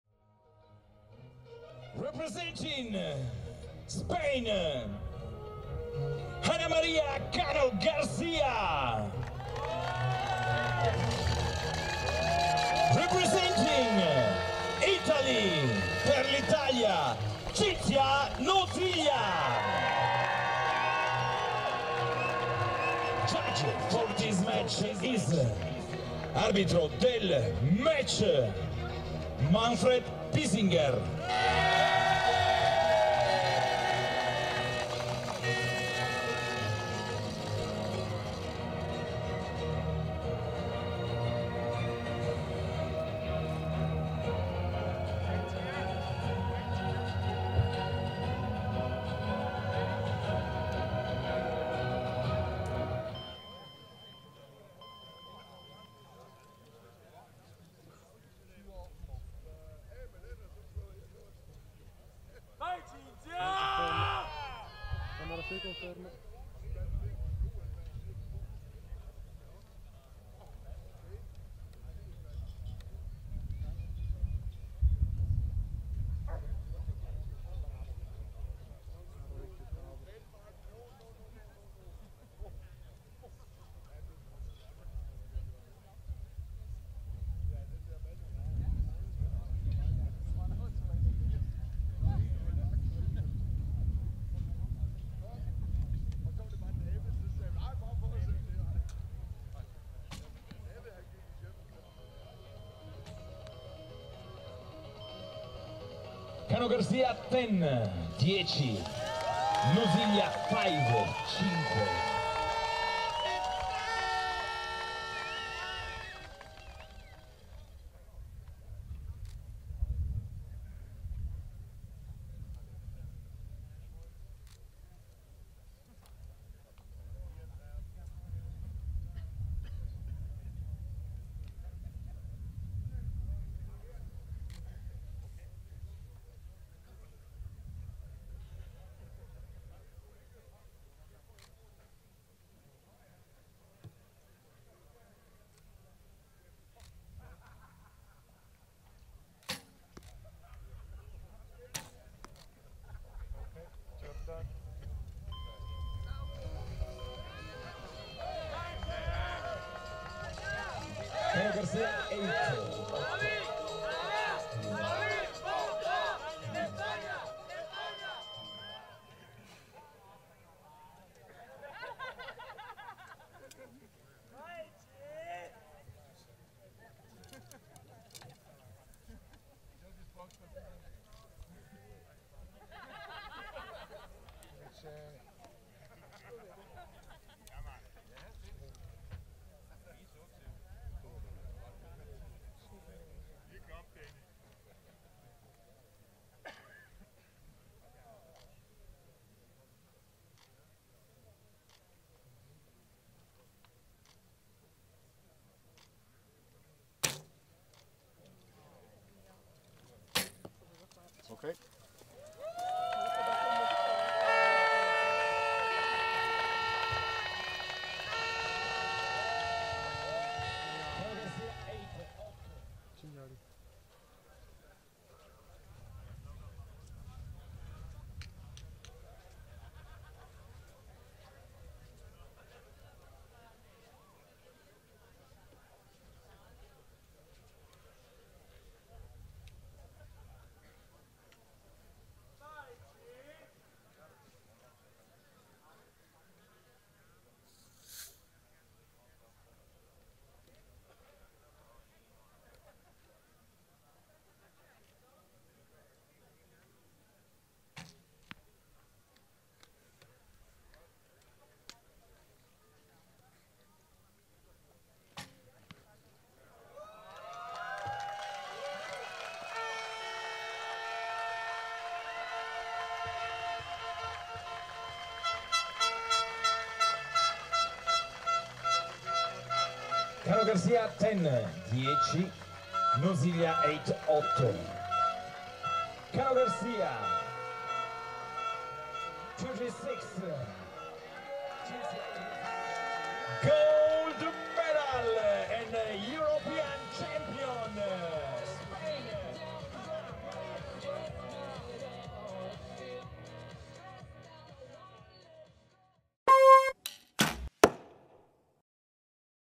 Ana Maria Cano Garcia v Cinzia Noziglia – barebow women gold | Cesana 2023 European 3D Championships
Barebow women gold match between Ana Maria Cano Garcia and Cinzia Noziglia at the 2023 European 3D Championships in Cesana–San Sicario, Italy.

and click on the bell 🔔 to get notified when new videos land.